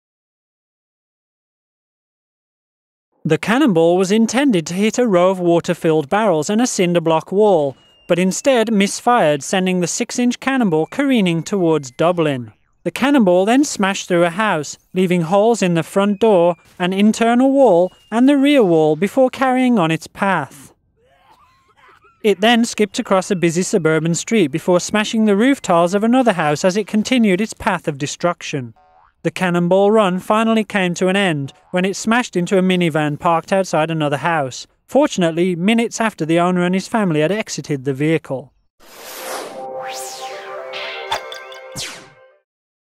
Mythbusters mishap: cannonball leaves trail of destruction
TV show Mythbusters was left with some explaining to do after an experiment it conducted to measure the speed of a cannon ball went wrong, leaving a trail of destruction across a California suburb. Instead of hitting it's intended target the cannon misfired, sending a six-inch ball of lead careening through one through one house, damaging another before ending up lodged in a minivan. No one was injured.